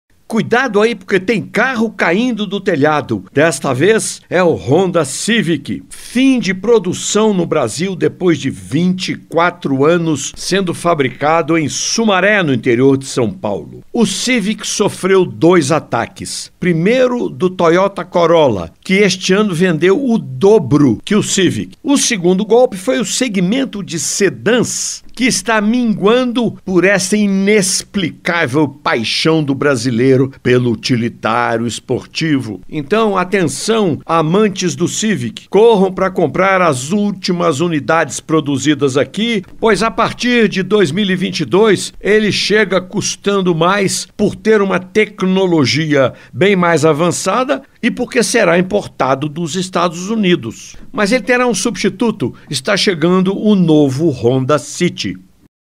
Civic recebe golpe duplo e cai do telhado
Após o sucesso no mercado nacional, o Civic perdeu espaço para concorrentes e a Honda anunciou que o sedan não será mais fabricado no Brasil.